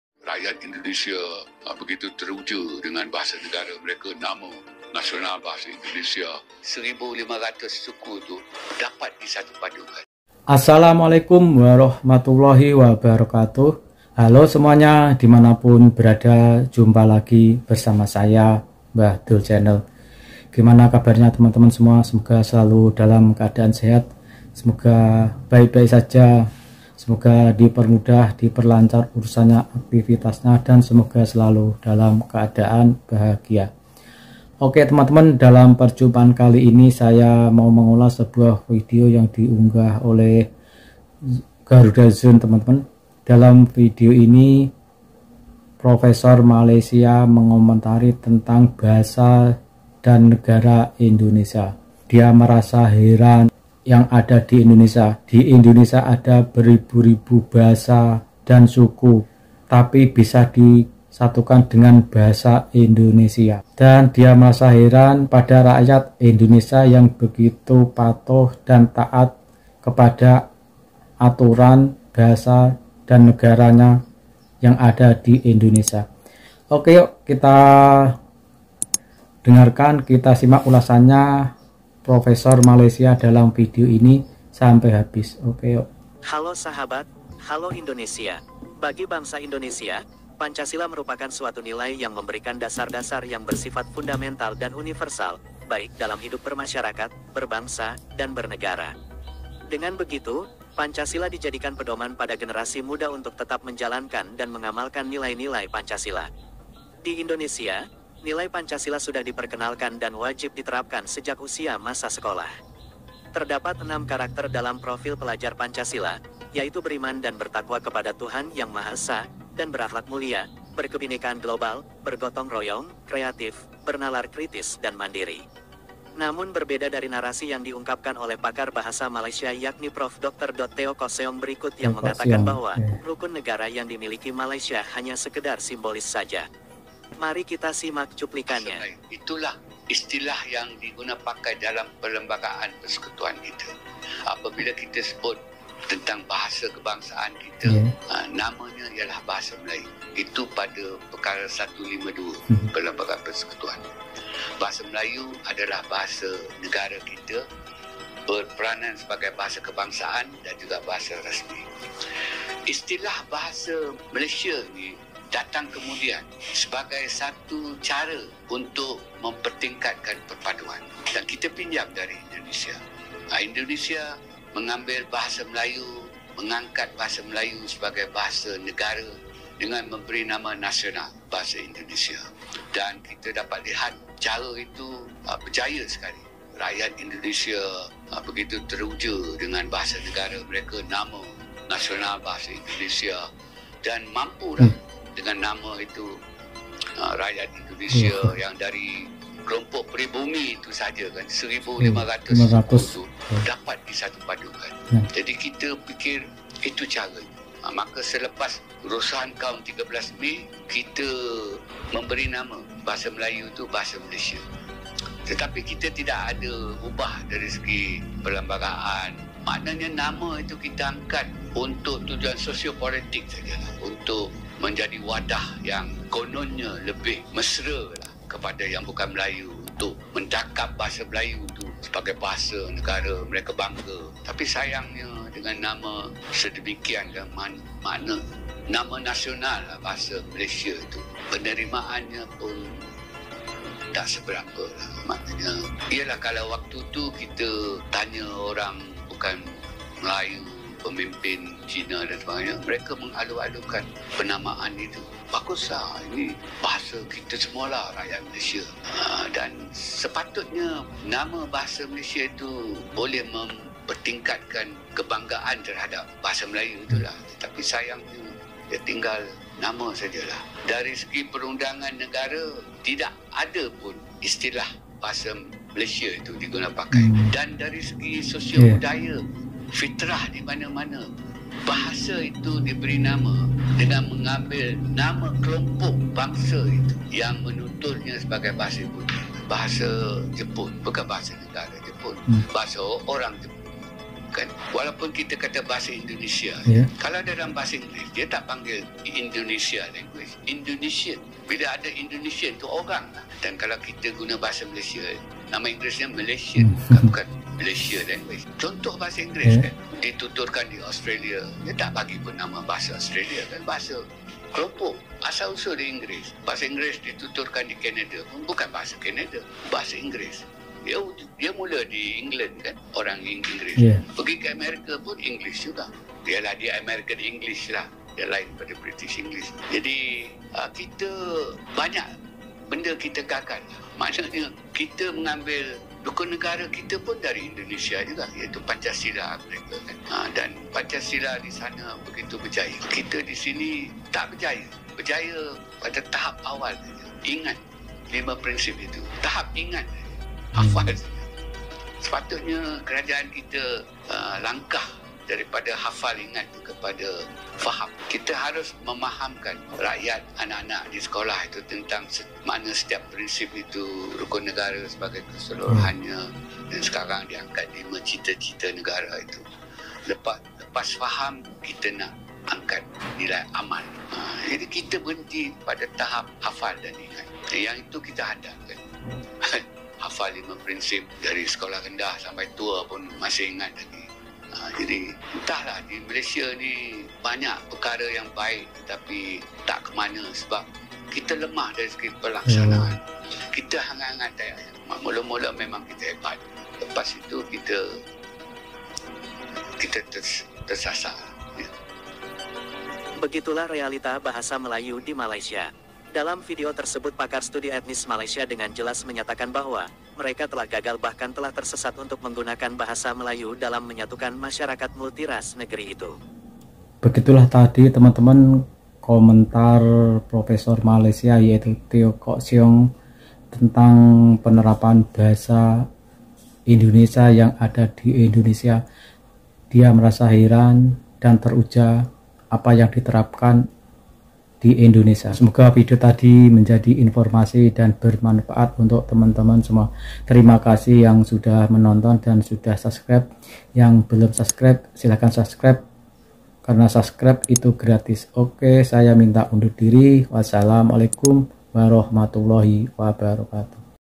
Prof. Malaysia Heran !! Ribuan Suku Dan Bahasa Bisa Disatukan Dengan bahasa Indonesia || 🇲🇨Reaction
Profesor Malaysia Heran,Ribuan Suku Dan Bahasa Bisa Disatukan Dengan satu bahasa Indonesia